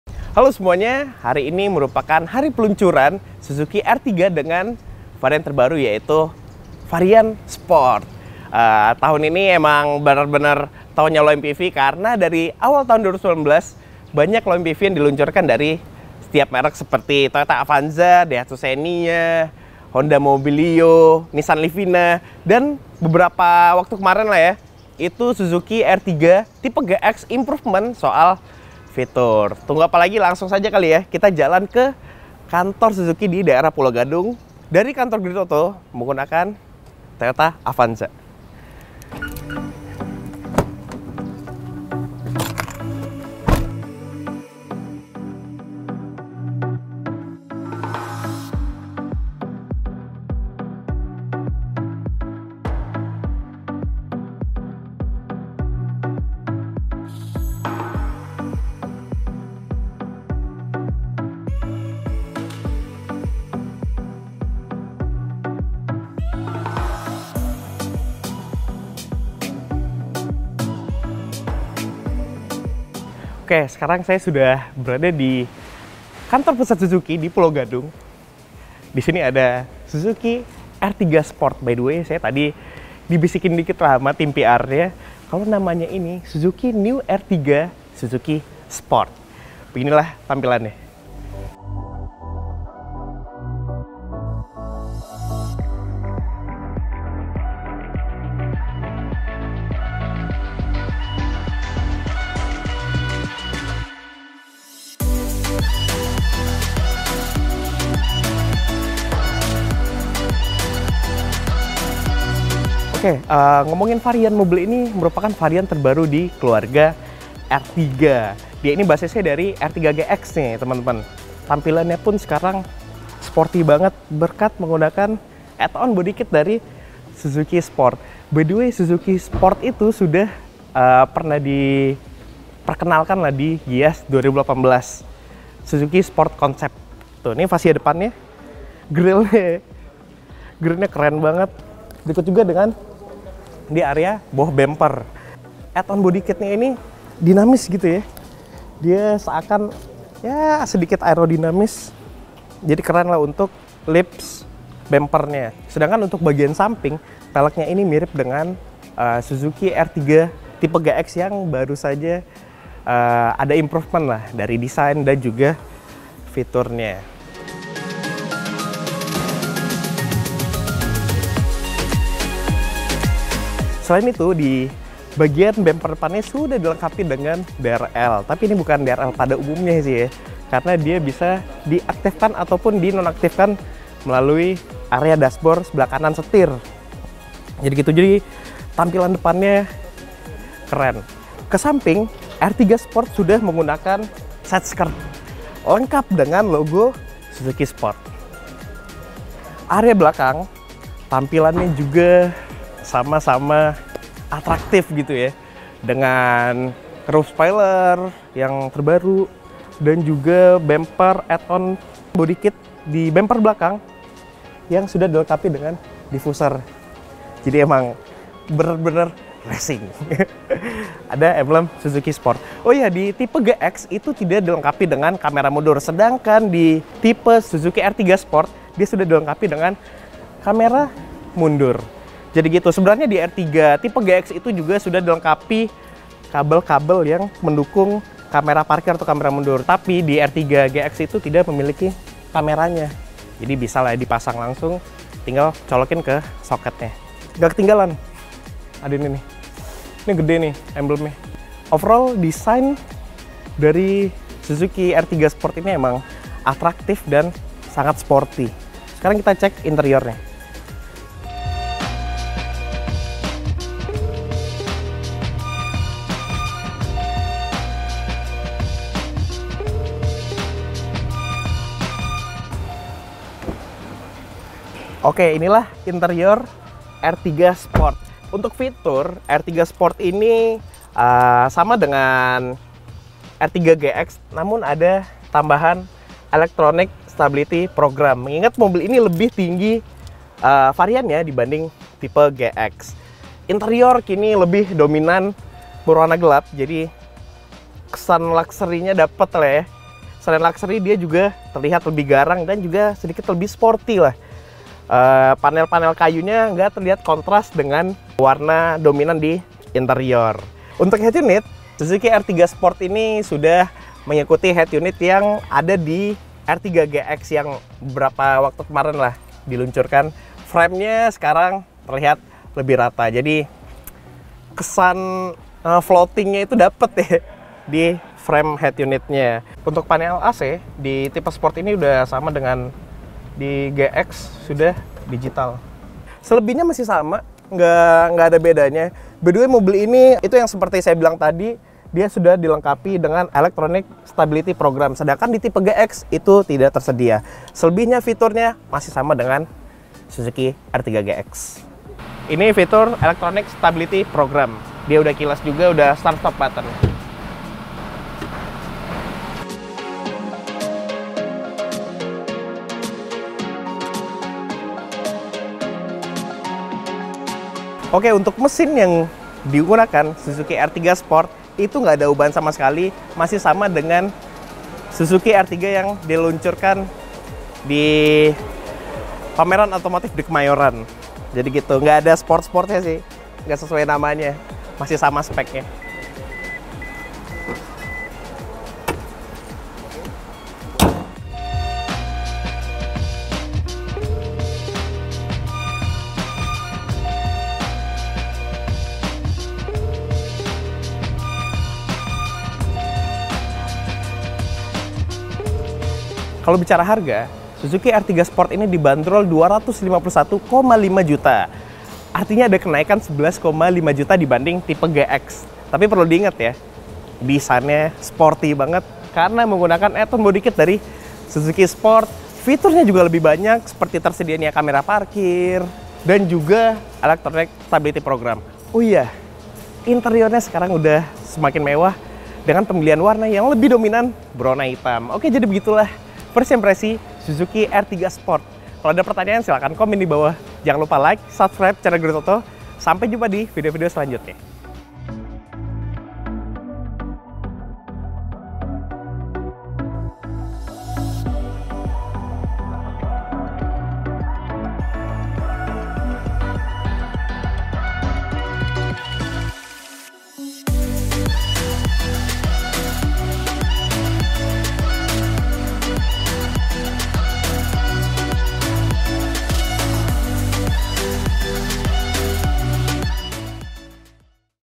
Suzuki Ertiga Sport 2019 | First Impression
Halo teman-teman.

Setelah melakukan improvement kecil-kecilan di Ertiga GX, Suzuki Indonesia kini menambahkan varian terbaru di keluarga Ertiga, dengan hadirnya Suzuki Ertiga Sport.

Tampilan Suzuki Ertiga Sport lebih aktraktif dan juga sporti, berkat add-on bodykit dari Suzuki Sport.

Tak hanya tampilan yang semakin ganteng, Ertiga Sport juga mendapatkan tambahan fitur terbaru.

Ingin tahu seperti apa perbedaanya jika dibandingkan dengan Ertiga GX?

Simak video First Impression Suzuki Ertiga Sport yang diulas Host/Test Driver GridOto Fikri Wahyudi

Selamat menyaksikan.

Jika Anda menyukai video ini, tekan tanda jempol ke atas. Terima kasih.